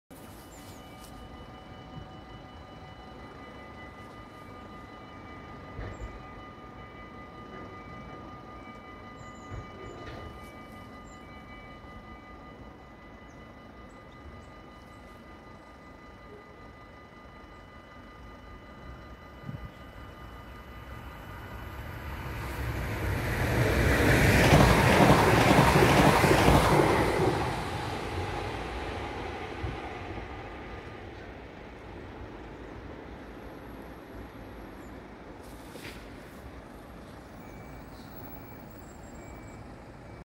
Spoorwegovergang Borne // Dutch railroad crossing
Locatie: Jupiterstraat
Traject: Almelo - Hengelo
Wacht borden: 6
Rode lichten: 10
Andreaskruisen: 4
Bellen: 4
Bomen: 6
Passeren:
FLIRT als SPR van Zwolle naar Enschede